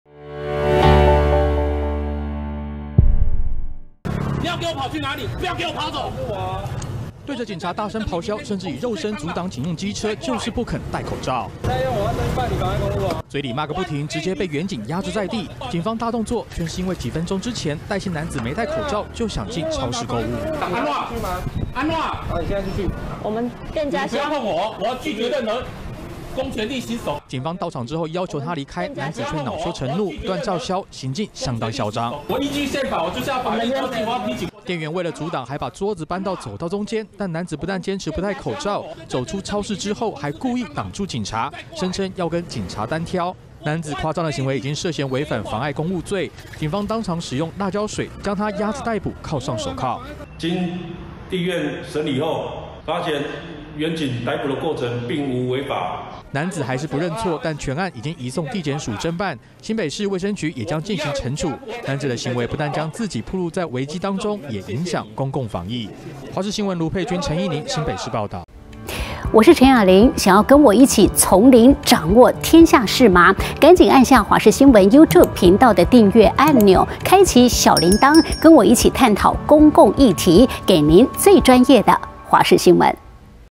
男子拒戴口罩飆罵警 肉身擋車遭壓制｜華視新聞 20220211
口罩已經是一般人出門必備，然而新北市有名男子，想到超市購物，卻拒絕戴口罩。警方到場後，還大聲飆罵。雙方走出店外後，男子又在警用機車前阻擋，並出言恐嚇。被警方當場依違反公共危險罪，以現行犯將男子壓制逮捕。